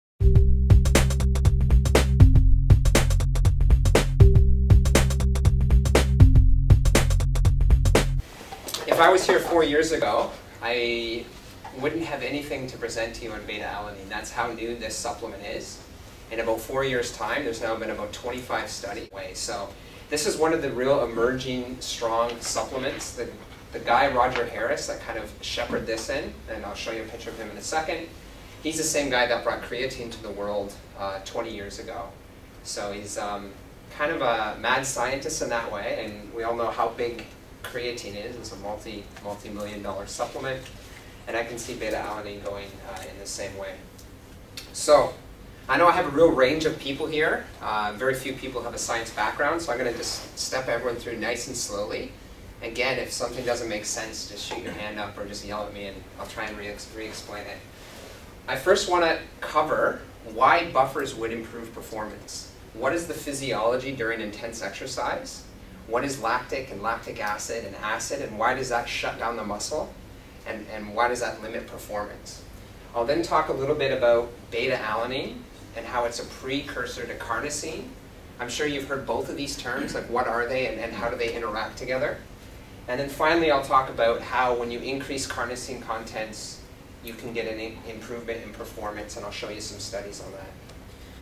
A Guide to Beta-Alanine Part 1 Introduction
Dr Trent Stellingwerff PhD is a sports scientist at the Nestle Research Centre in Vevey, Switzerland. Trent presents an overview of the latest sports supplement to hit the market,  Beta-Alanine.

Touted as the next big thing since Creatine, Dr Trent takes us through the thinking behind Beta-Alanine and the associated studies that show the impact of Beta-Alanine on performance.

In this video filmed at Musashi HQ, Dr Trent show us that a sustained release Beta-Alanine has a higher absorption rate than the pure supplement.